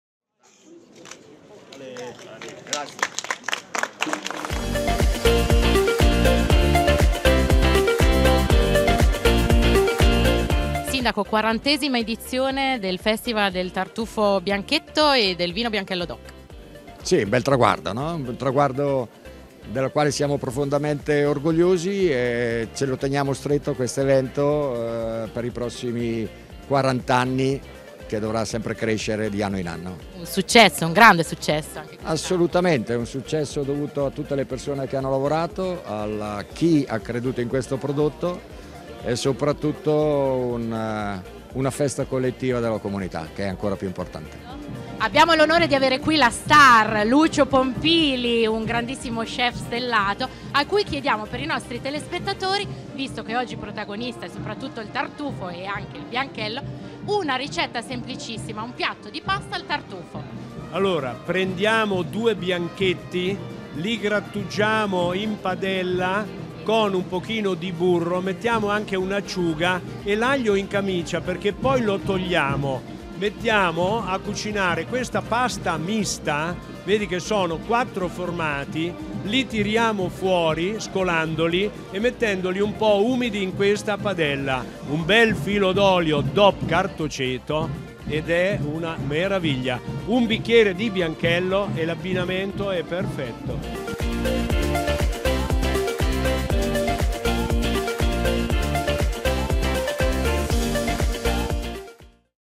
Fossombrone, tartufo e bianchetto: al via il festival 2024
Chef stellati, degustazioni, iniziative collaterali e novità. A Fossombrone inaugurata la 40^ edizione del Festival del Tartufo Bianchetto e del Vino Bianchello. Si replica il 23 e 24 marzo.